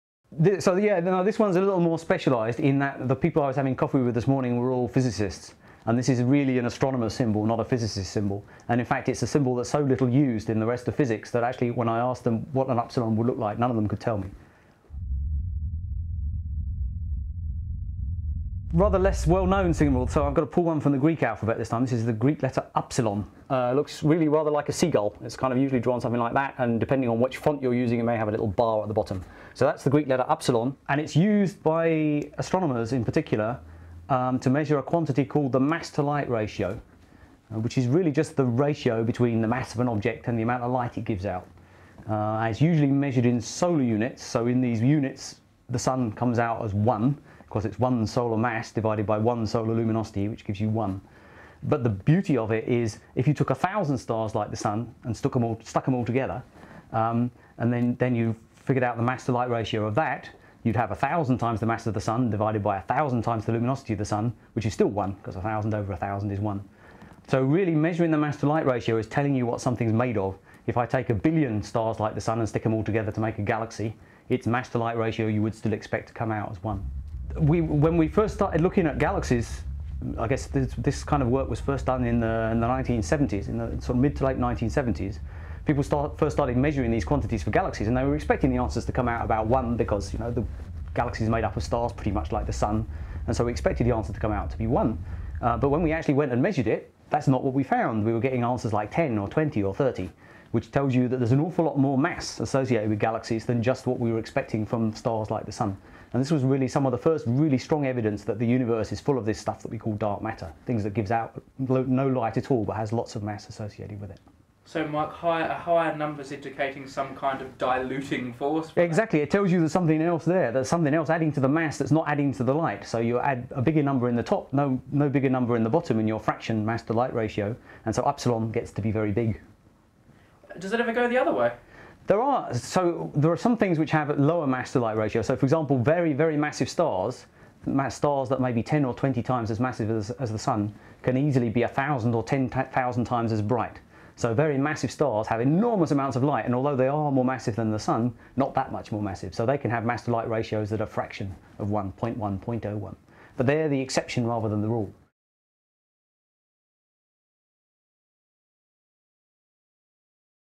Mass to Light Ratio - Sixty Symbols
It's a funny symbol that looks like a seagull - but it helps astronomers learn what galaxies are made of.

Featuring Mike Merrifield.